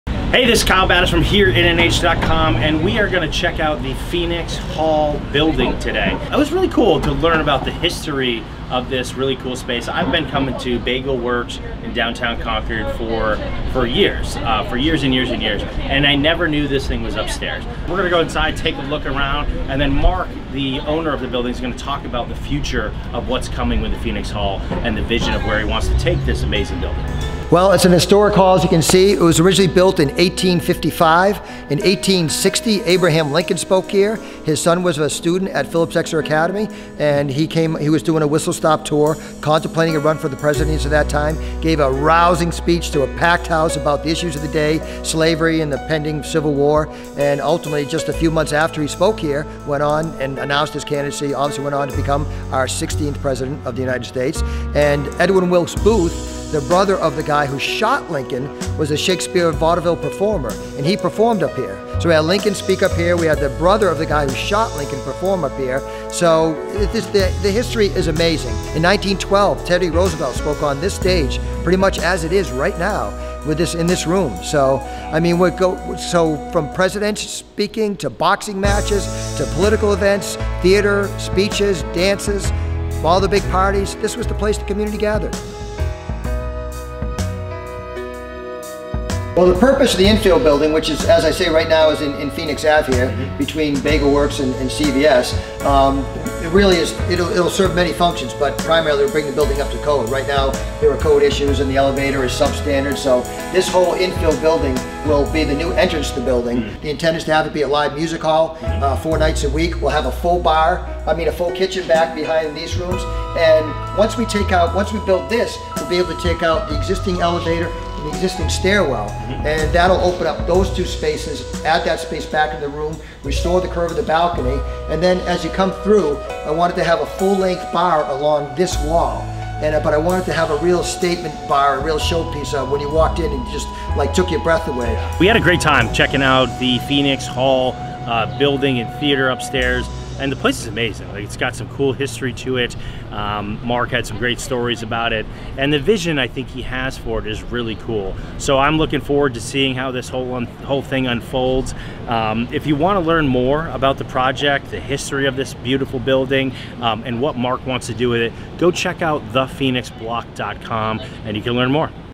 Phenix Hall Concord NH
Concord NH - For years living in Concord I heard rumors of a unique Theater above 'The Works' but I had no idea the history of this grand old hall until recently.  I was surprised to learn that it can seat 499 people and even more impressed with stories of what happened in this space in the past.  I had the opportunity to speak with building owner Mark Ciborowski and he revealed the history of this amazing Theater while also sharing his plans on bringing the Phenix Hall back from the ashes.